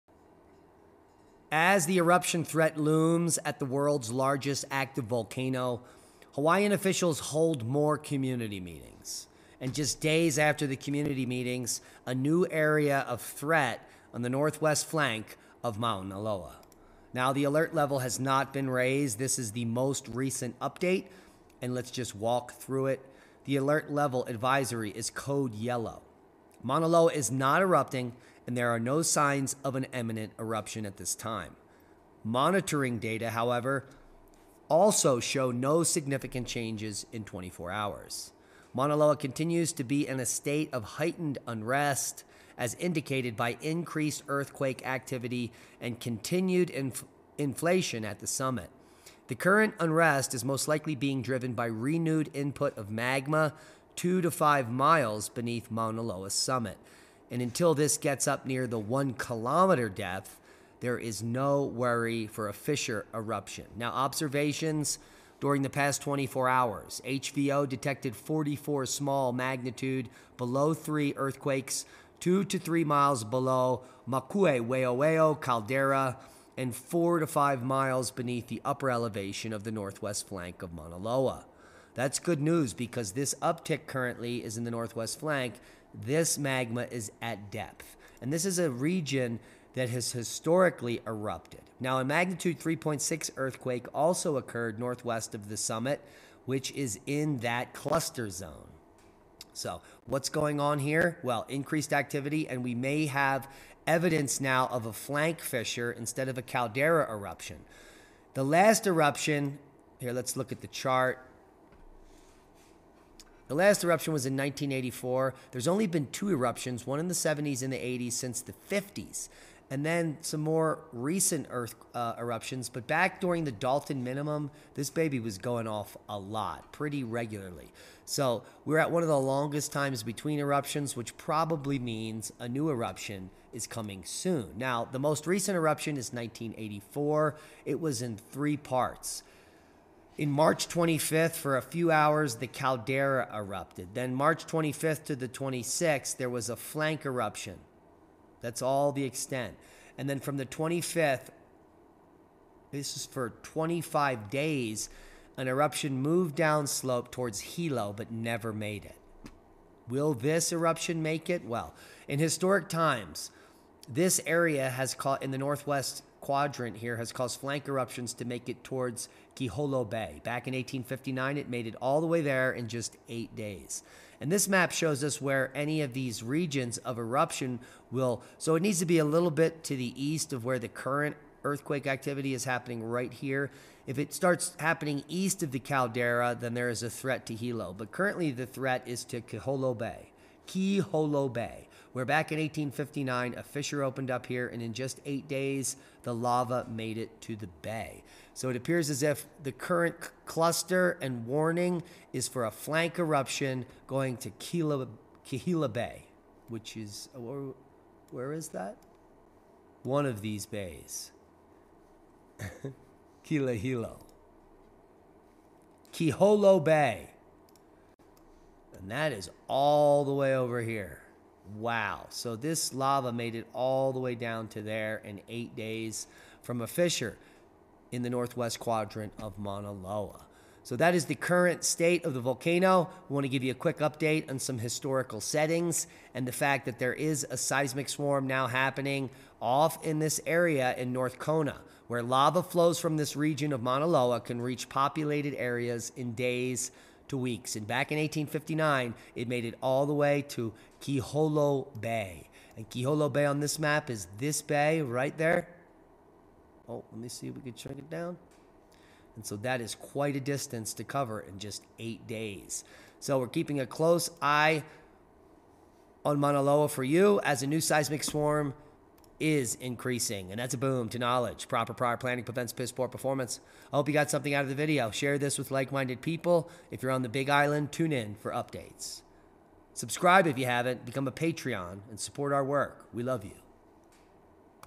Eruption Threat Looms at World’s Largest Active Volcano - Seismic Swarm In The Northeast Kona Region
As Eruption Threat Looms at World’s Largest Active Volcano, Hawaii Officials Hold More Community Meetings
HAWAIʻI ISLAND - Mauna Loa is not erupting, although the volcano continues to show signs of heightened unrest.

Bitcoin: 17NV3vtcQhRnsRnjckCcYkKVEUcnirKQ62

Bitcoin: 17NV3vtcQhRnsRnjckCcYkKVEUcnirKQ62